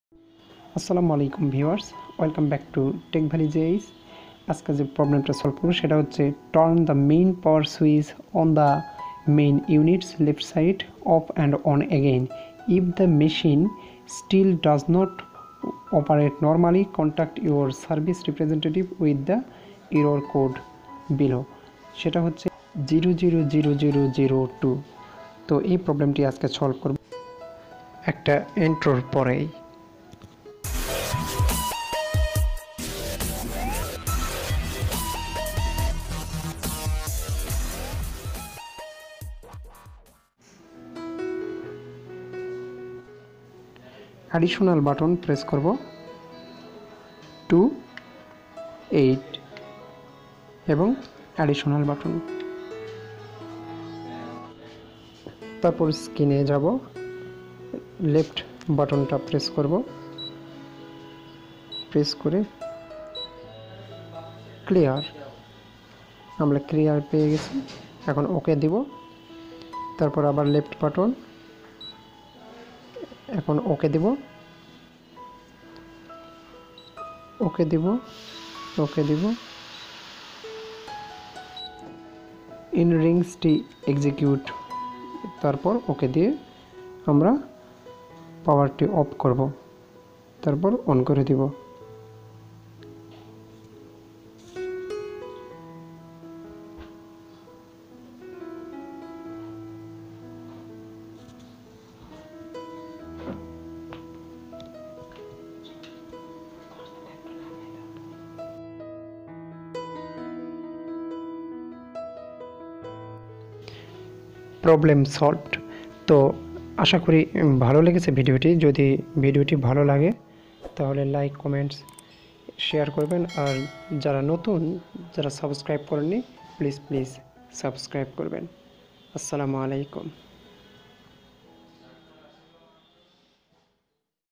Turn The Main Power Switch Canon ImageRunner 2535, 2525 E000001, E000002 error Solution
Turn The Main Power Canon ImageRunner 2535, 2525 E000001, E000002 error Solution